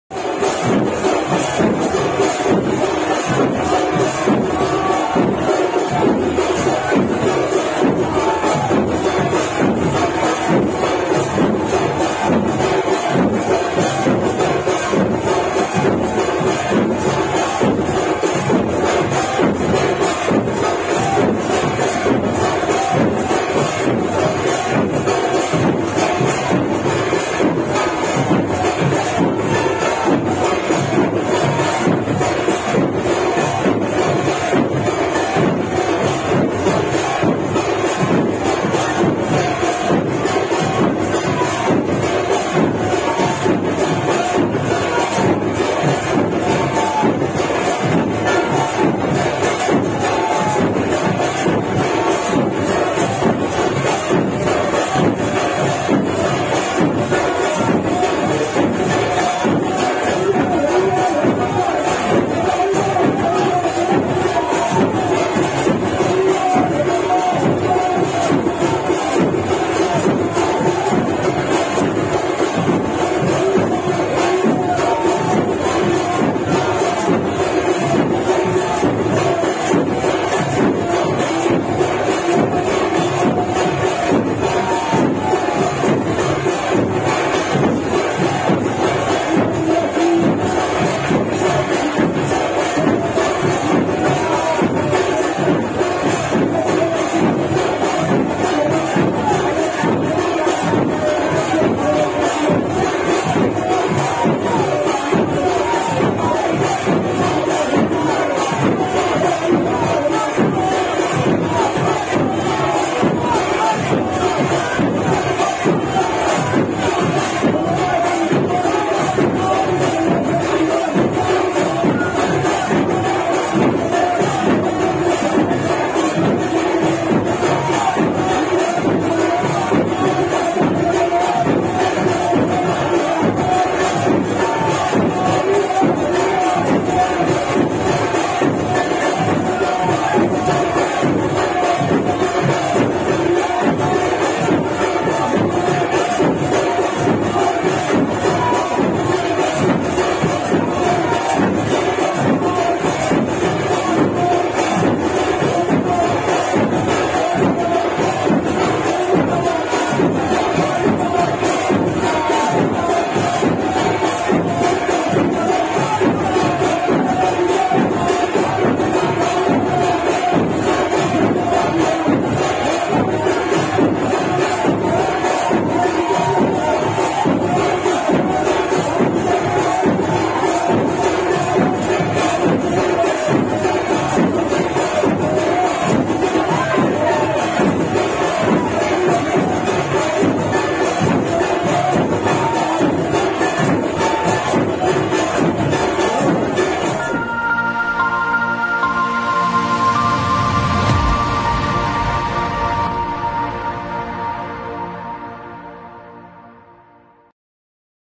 The fans of Harichand Tagore are going crazy with Matua Matam in Nagarkanda of Faridpur
Channel: Jago Matua 🚩🚩
Admin & Editing: Akash Bala
Also like this video and stay with us for more exclusive video.